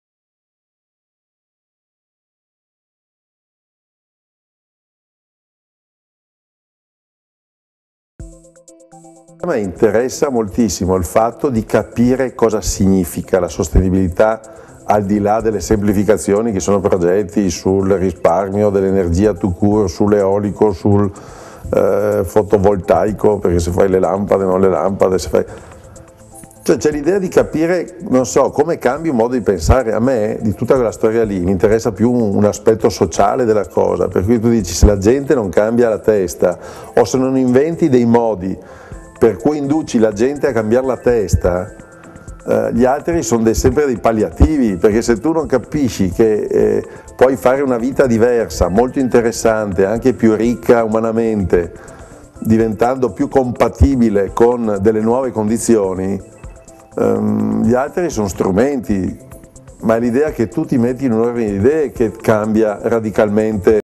Alfa Romeo Lectures: Aldo Cibic - design e designer
Alfa Romeo Lectures: Aldo Cibic sul design ed essere designer (Aldo Cibic speaks about his design and beeing a designer)